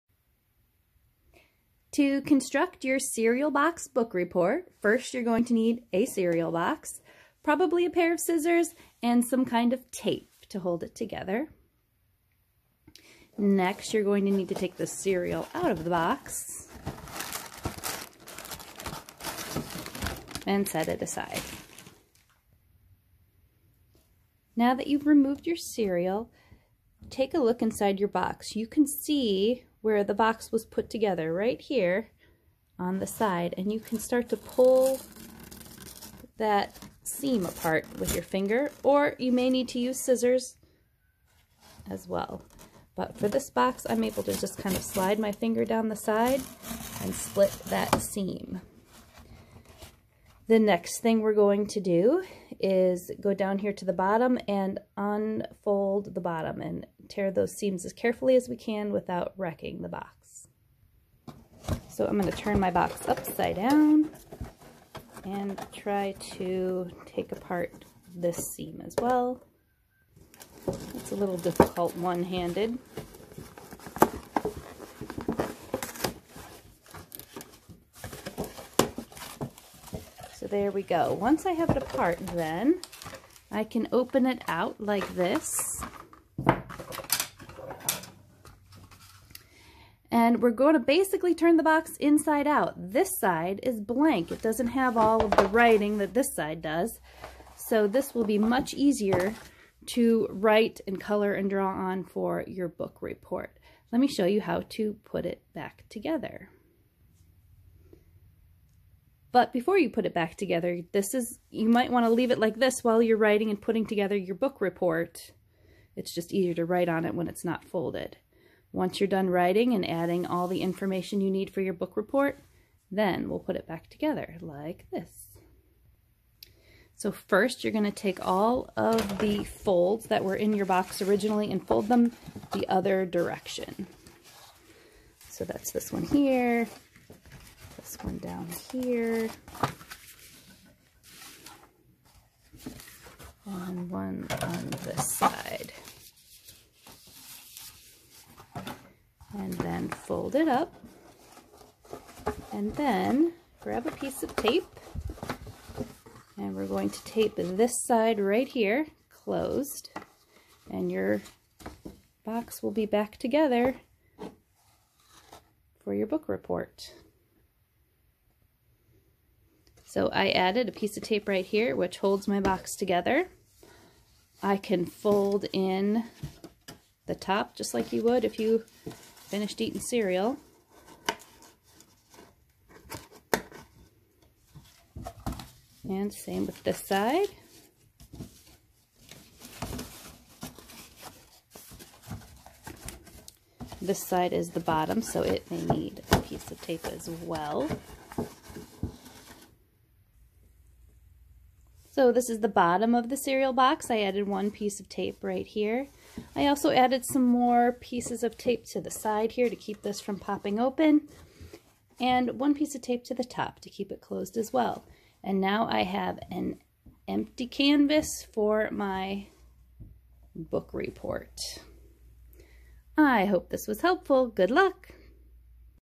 Cereal Box Book Report: Simple Construction
How to easily make and create your cereal box book report for school. Very Simple!